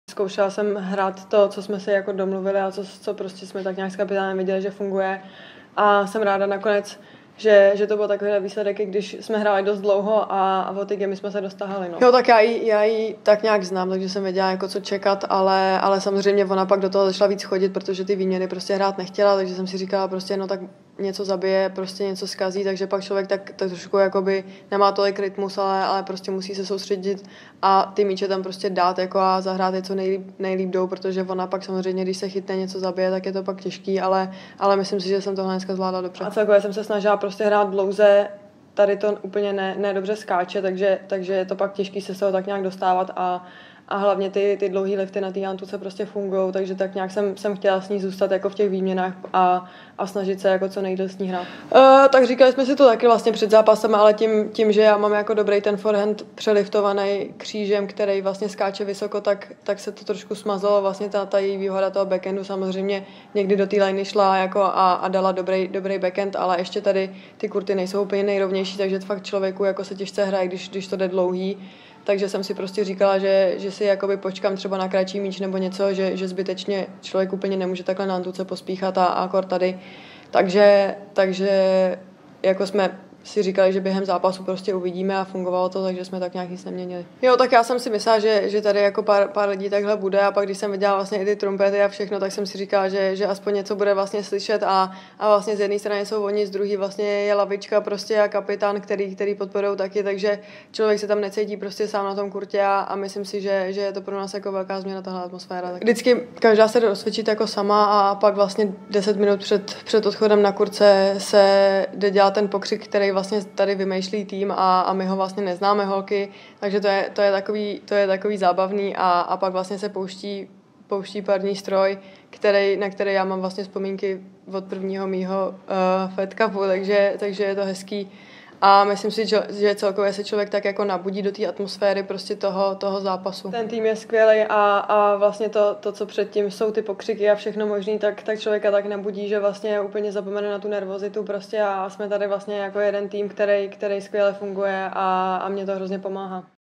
Markéta Vondroušová po výhře nad Martou Kosťukovou | BJKC 2023 (kvalifikace)
Kvalifikace Poháru Billie Jean Kingové, Belek (Turecko): Marta Kosťuková (UKR) – Markéta Vondroušová 2:6, 1:6